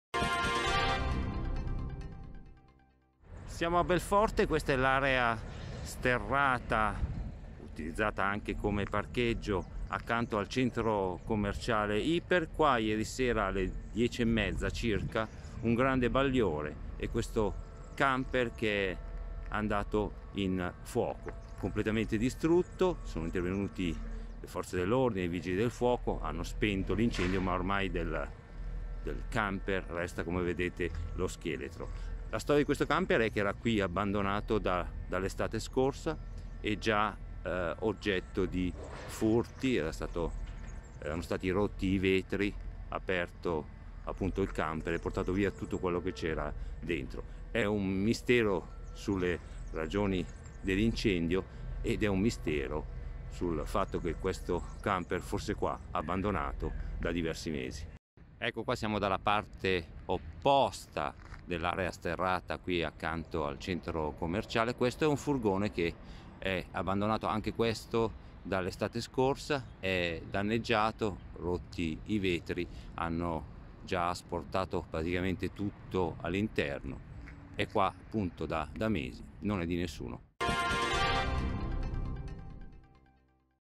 Varese - Camper 25/3/2021
Il giallo dell'incendio vicino all'Iper (Martinoli)